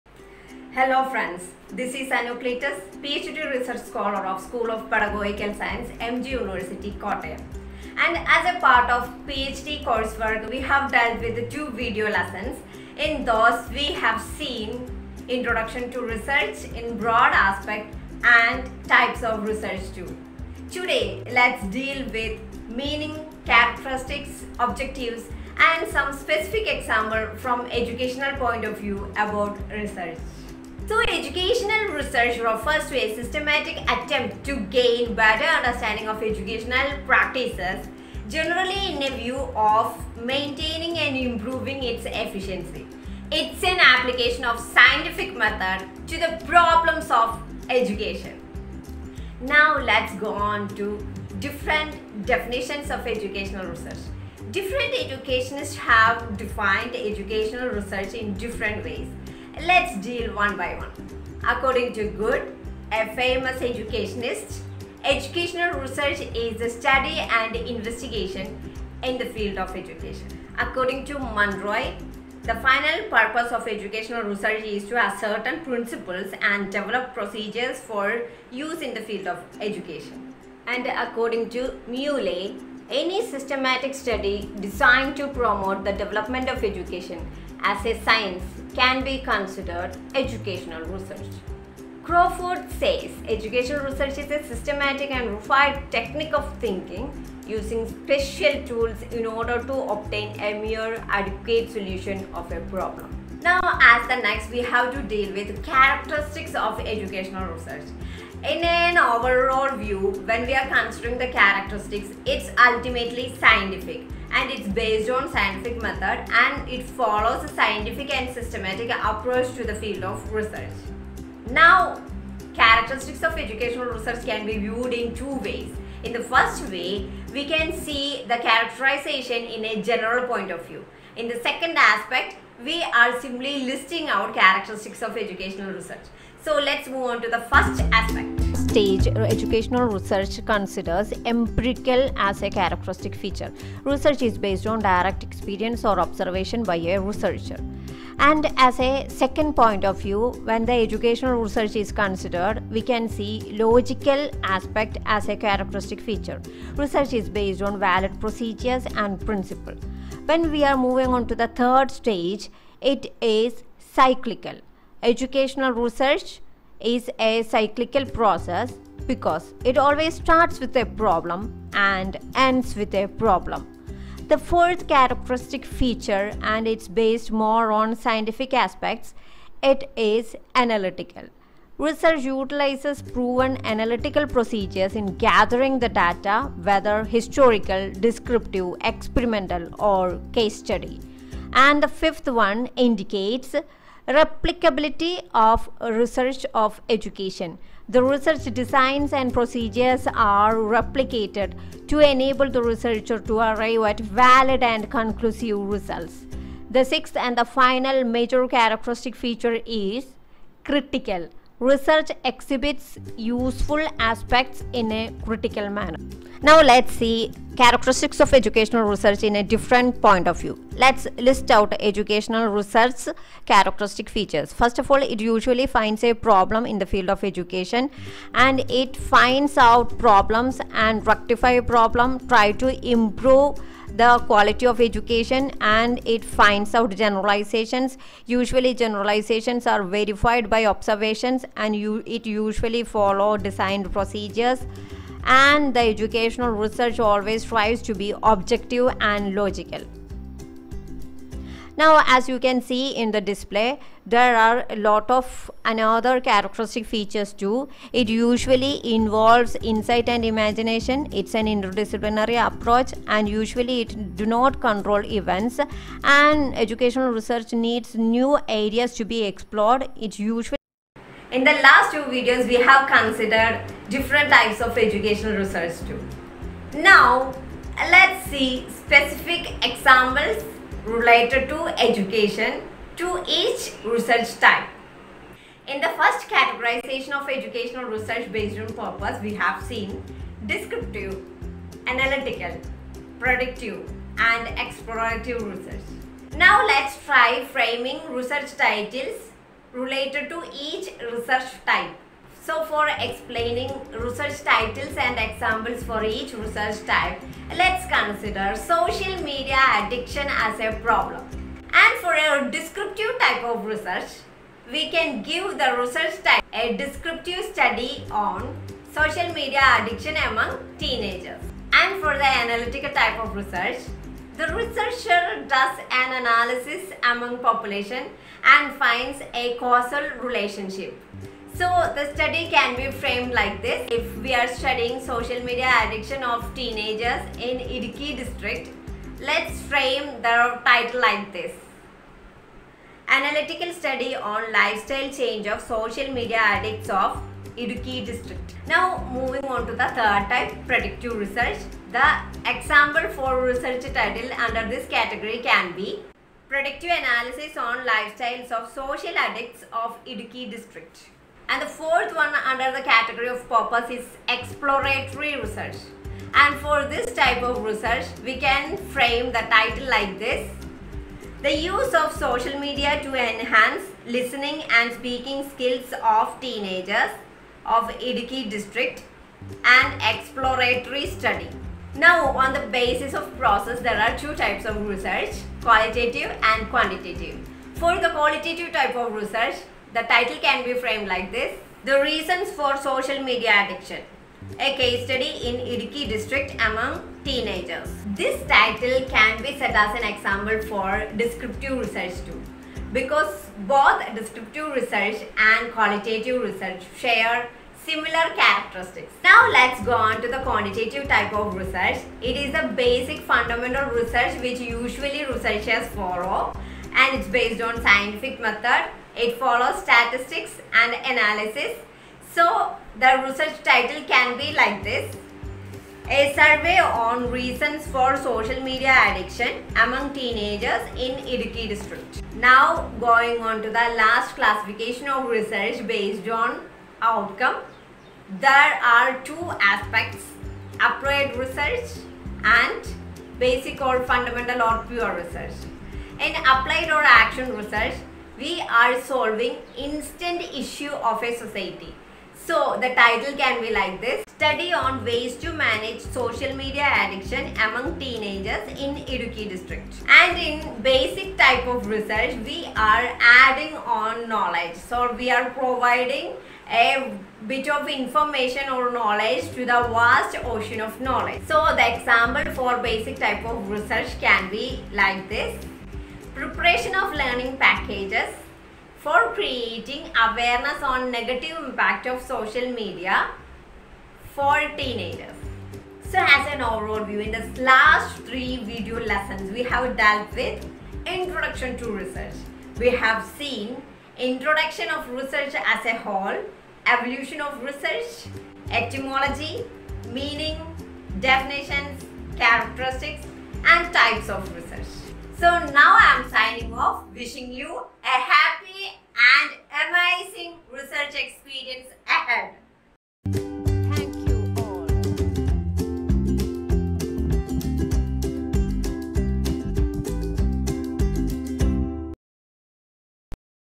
MEANING|DEFINITION|CHARACTERISTICS OF RESEARCH | RESERACH TITLES |PhD COURSE EDU P1 |VIDEO 3
The video maker is Anu Cleetus, educator by profession  and researcher by passion. The video is made as a part of PhD course work and the e content details meaning, definitions, characteristics and examples for research types -descriptive,analytical,predictive,exploratory,qualitative,quantitative,applied and basic researches. THE VIDEO IS THE 3RD VIDEO WITH THIS REGARD
Introduction to research
MEANING OF RESEARCH
DEFINITIONS OF RESEARCH
CHARACTERISTICS OF RESEARCH
RESEARCH TITLES-EXAMPLES